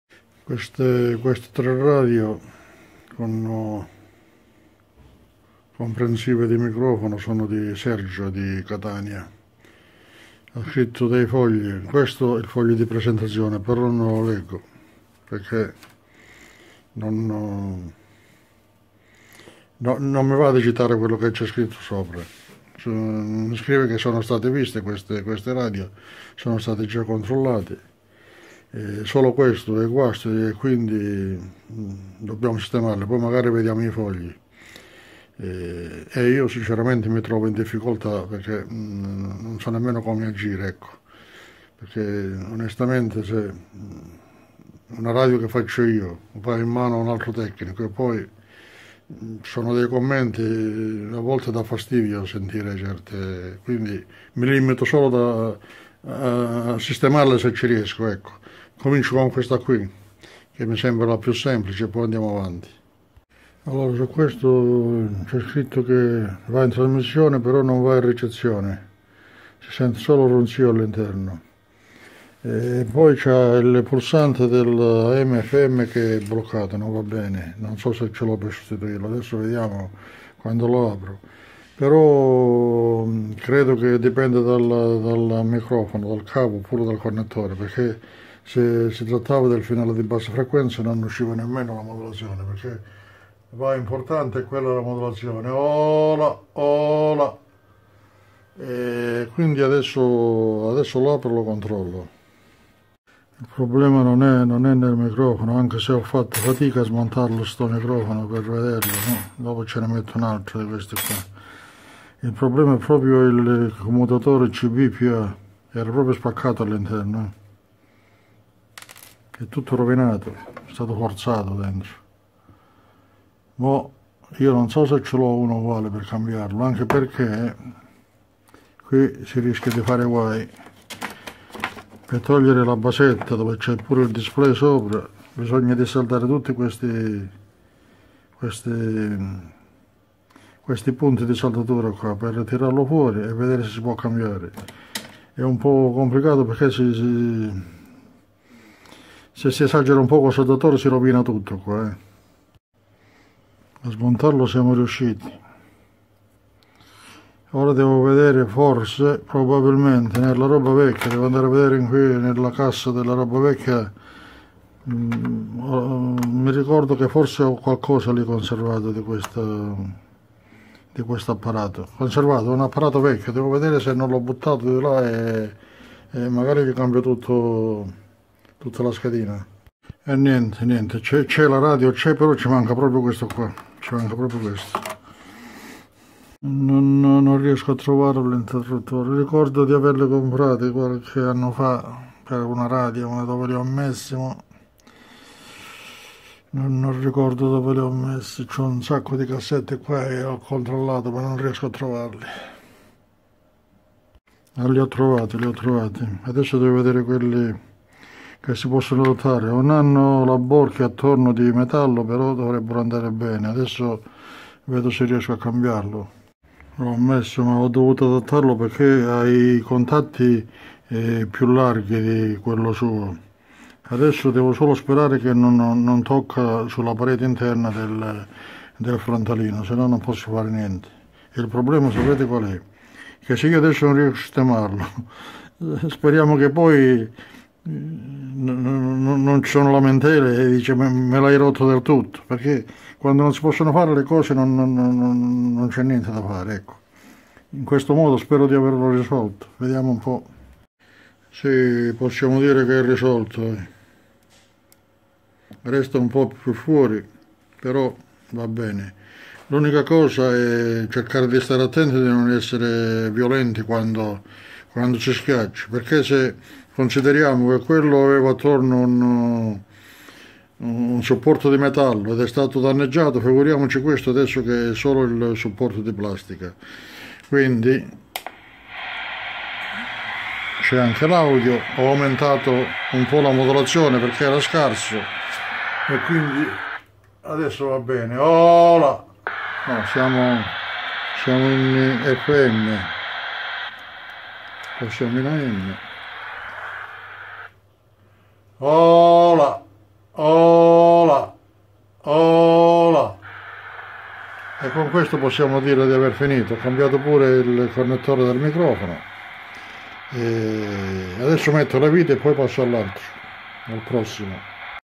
Riprese di tre apparati CB di Sergio di Catania - Ricondizionati -
l primo un Midland 77-114, gli altri in riprese a parte.